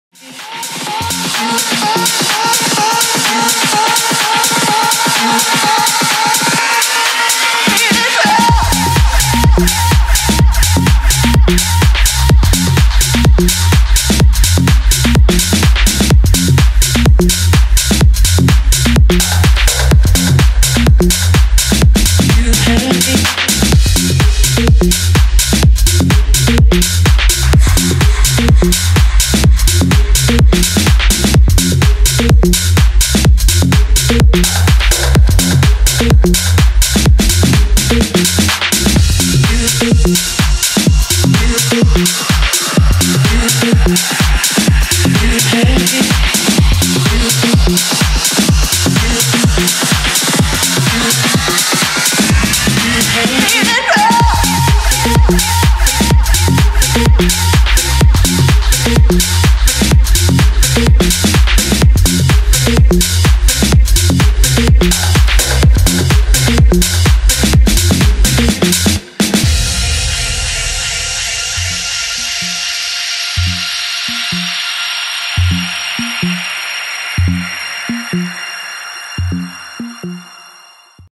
Black Legend @ Jango Closing - Bora Bora Ibiza 27/8/2015
A summary of my gig at the Jango Music season closing party at  Bora Bora Ibiza